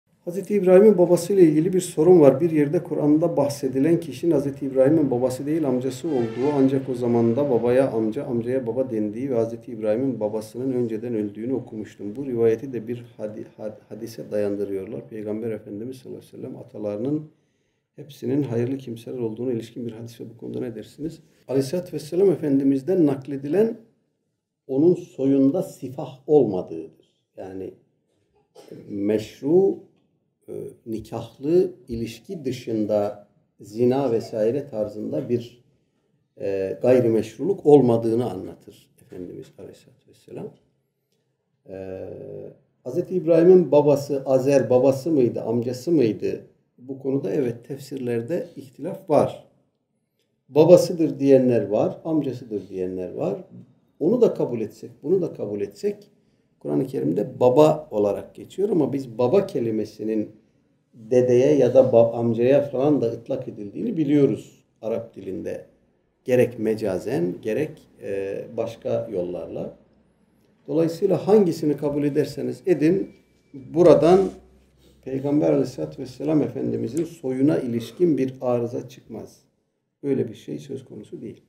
Ebubekir Sifil - Hz. İbrahim (as)'ın Babası Azer Değil Miydi?
Bismillah.

Kıymetli Kardeşlerimiz,

Ebubekir Sifil hoca 14 Mayıs'ta gerçekleştirmiş olduğu İslam ve Hayat Sohbeti'nde katılımcımızdan gelen bir soru cevaplandırmakta.

{Soru}

Hz. İbrahim babasıyla ilgili bir sorum var. Bir yerde Kur'an'da bahsedilen kişinin Hz. İbrahim'in babası değil amcası olduğu ancak o zamanda amcaya baba dendiği ve Hz. İbrahim'in babasının önceden öldüğünü okumuştum.

Bu rivâyeti de bir hadise dayandırıyorlar. Peygamber Efendimiz'in (sav) atalarının hep hayırlı kimseler olduğuna ilişkin bir hadis var. Bu konuda ne dersiniz?

İlginiz için teşekkür ederiz.

Saygılarımızla,
Sahn-ı Semân Medya.